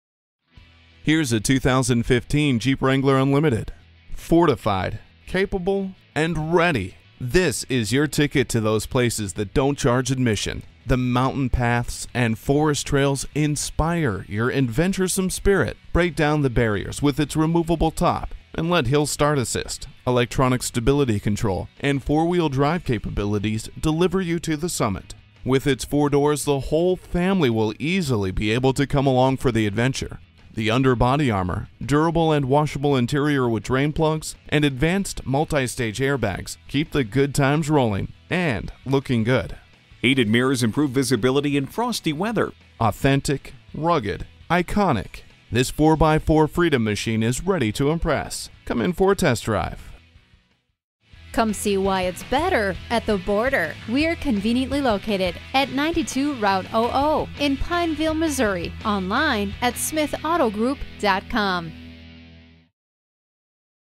2015 Jeep Wrangler Pineville MO Bella-Vista AR, MO #415288 - SOLD
Year: 2015
Make: Jeep
Model: Wrangler
Trim: Unlimited Rubicon
Engine: 3.6L V6 24V VVT
Color: Yellow
Stock #: 415288
VIN: 1C4BJWFG1FL558654
4X4! ATTENTION!!! Want to stretch your purchasing power? Well take a look at this stunning 2015 Jeep Wrangler. Neat little fuel-efficient ride here, folks! No trip is too far, nor will it be too boring! "It's better at the border" Come see why for yourself!

Address:
92 Route OO